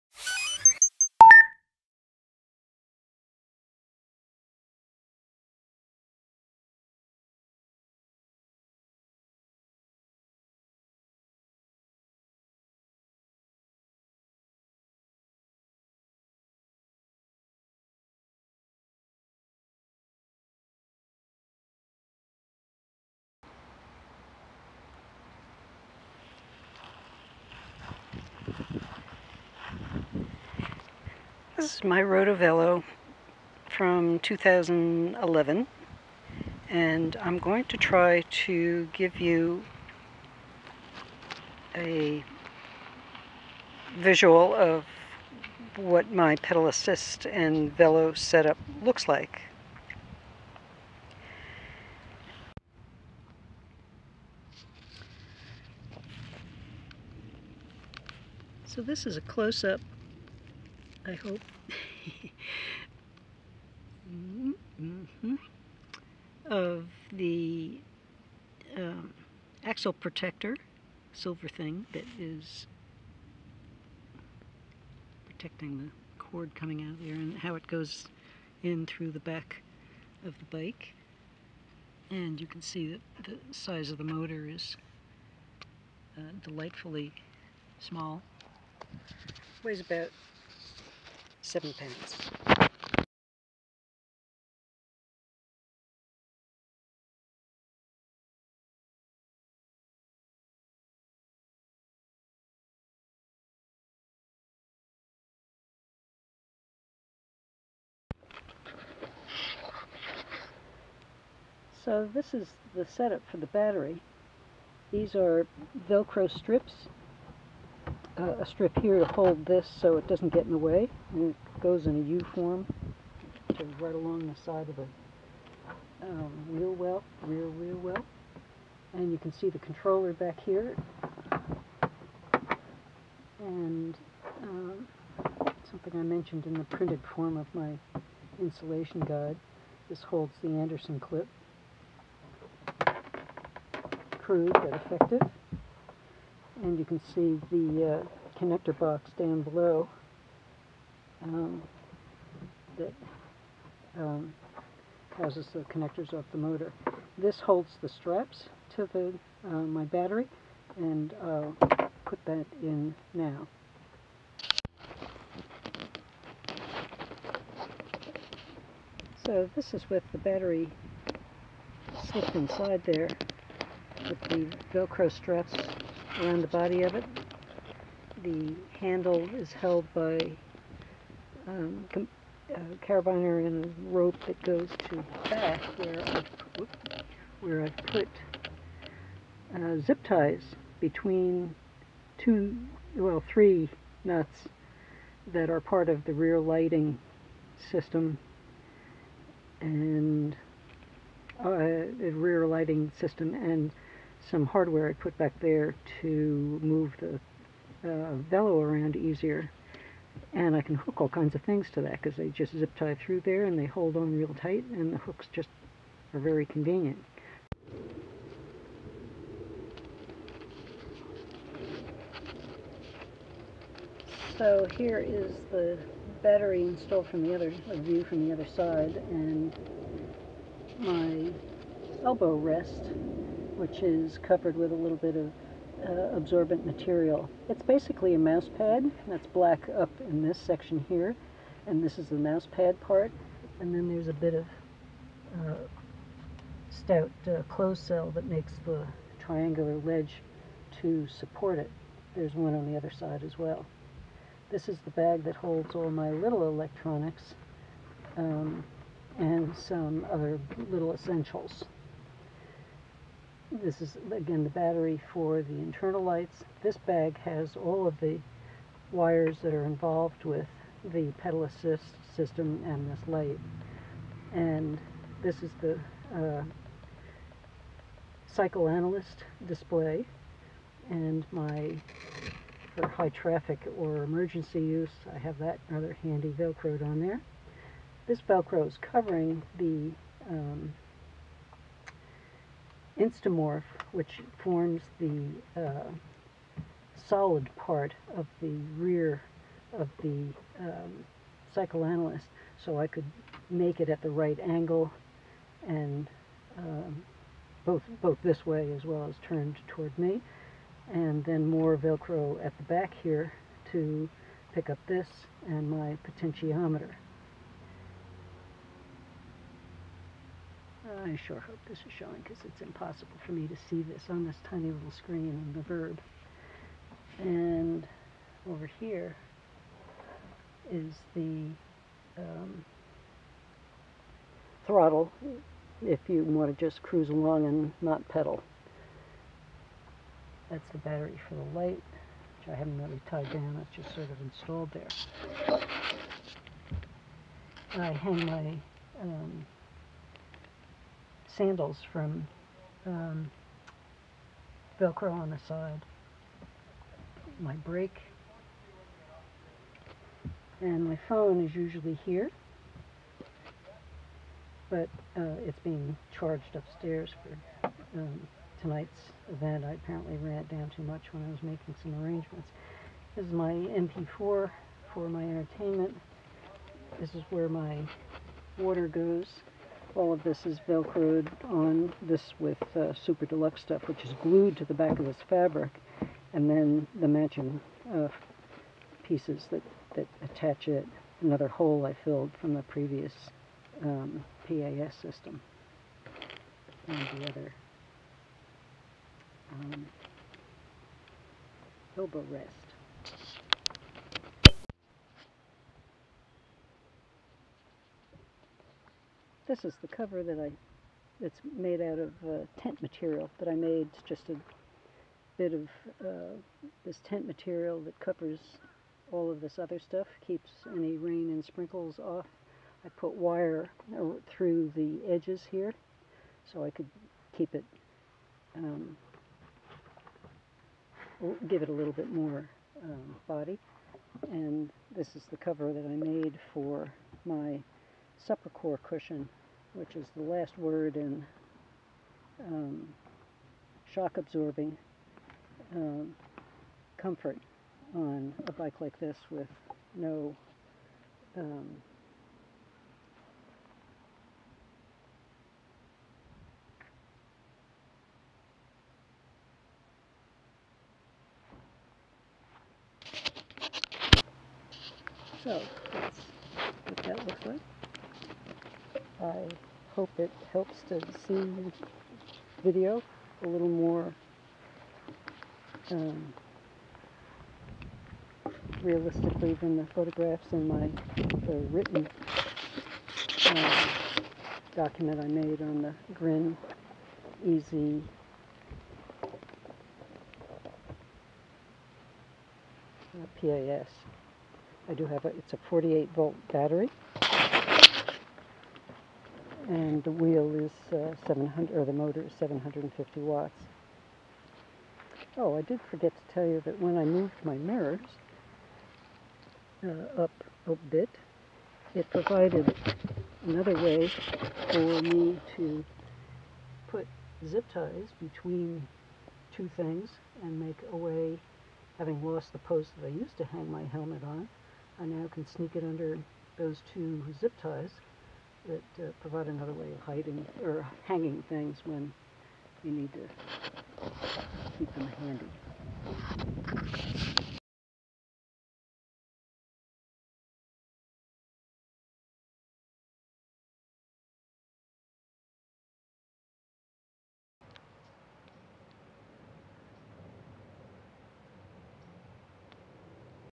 eZee PAS kit install on a RotoVelo
This supplements written a document that is an installation guide of the Grin Technologies eZee pedal assist kit.